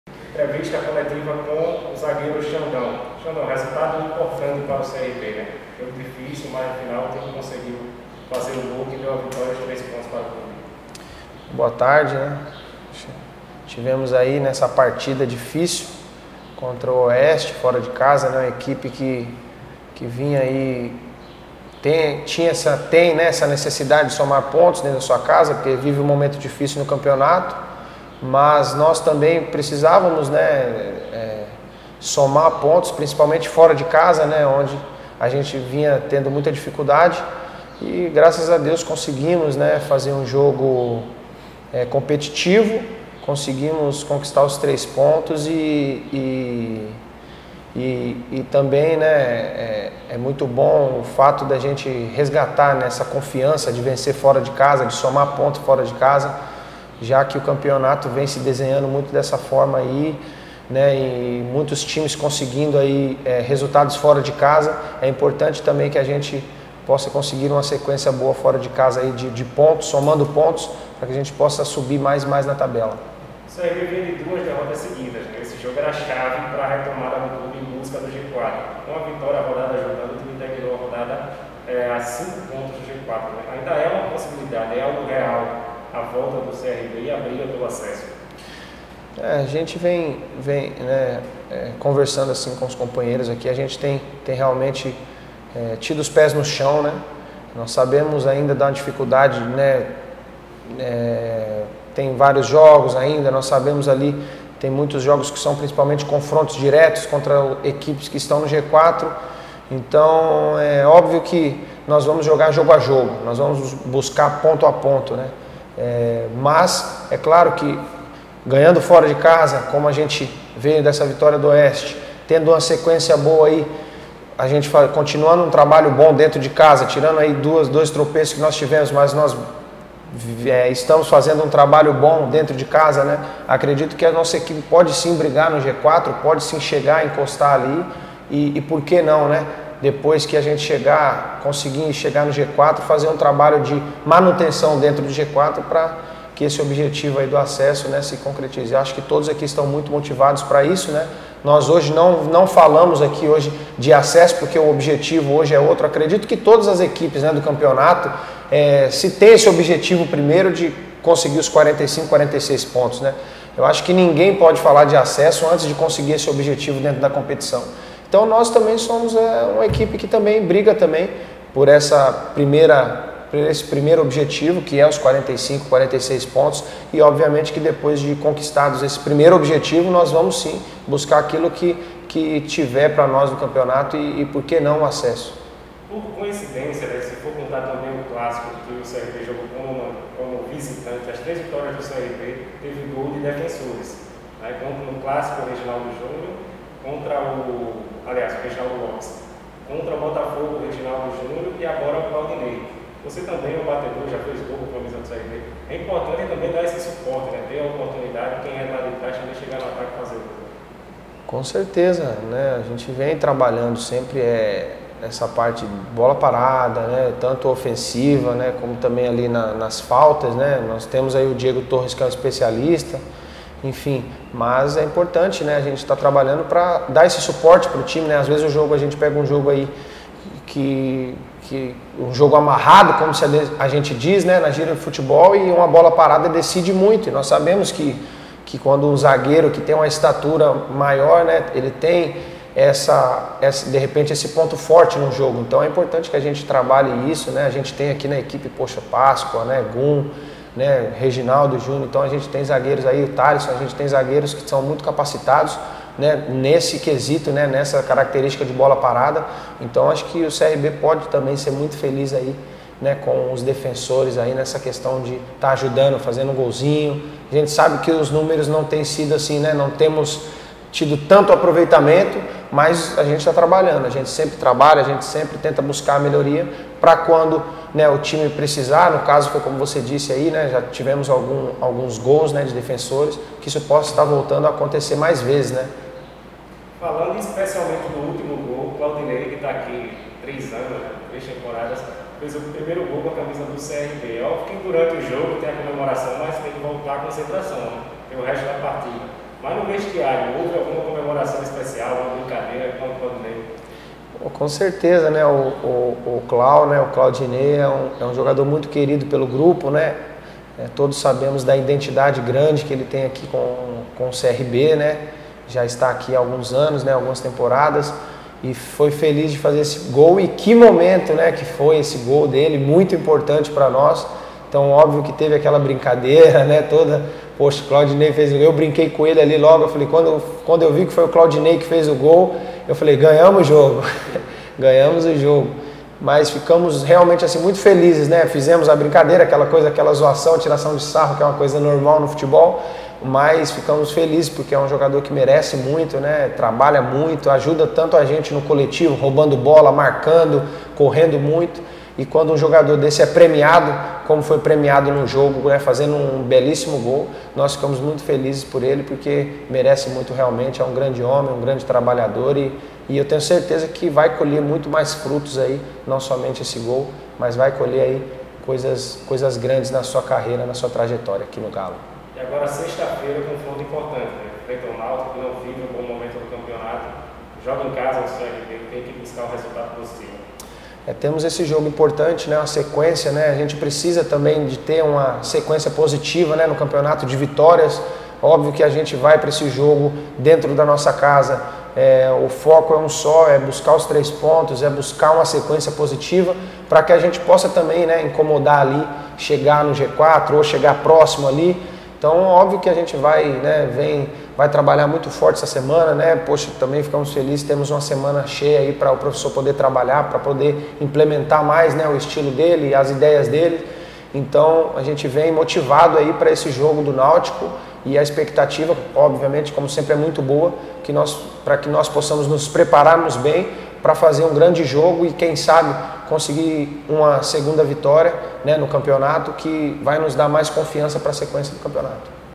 Entrevista Coletiva Xandão
Entrevista do zagueiro Xandão
CRB segue em preparação para enfrentar o Náutico no próximo sábado
Entrevista produzida pela ASCOM CRB